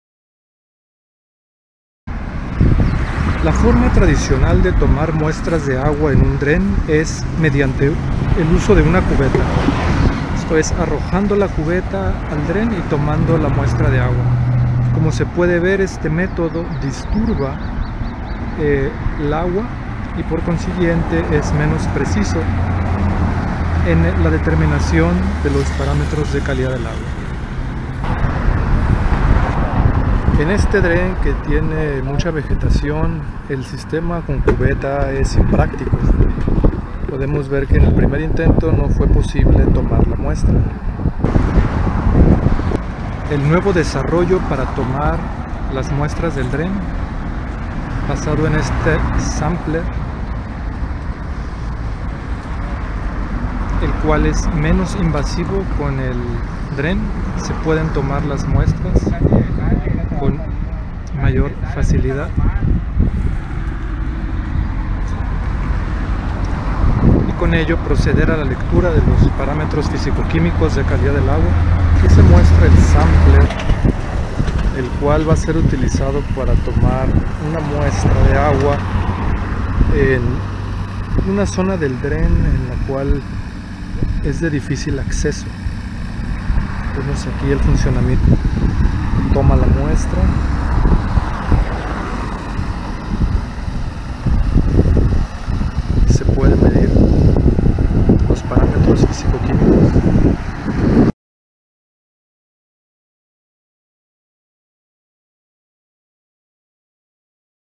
Filtro biológico natural (drenaje pluvial con vegetación) toma de muestras
El dren urbano con vegetación ubicado en el paseo del atlántico en la ciudad de Mazatlán es un ejemplo de infraestructura verde en Sinaloa, México. La presencia de vegetación en el dren permite el hábitat de organismos que encuentran refugio, descanso o alimento en este sitio.

La importancia ecológica de este sitio en el mejoramiento de la calidad del agua ha sido objeto de estudio de estudiantes e investigadores de la Facultad de Ciencias del Mar de la Universidad Autónoma de Sinaloa. Las coordenadas del sitio son: 23°16'20.810" N, 106°26'54.163" W. El video fue grabado en agosto 2017.

Detalles del instrumento para la toma de muestras "Sampler" (por su palabra al inglés) pueden ser encontrados en García-Pazos, J. (2017). Estudio exploratorio de la calidad del agua en escorrentías transportadas por el dren urbano atlántico (Mazatlán, Sinaloa). Tesina de licenciatura. Unidad Académica de Ingeniería en Biotecnología. Universidad Politécnica de Sinaloa.

Saludos!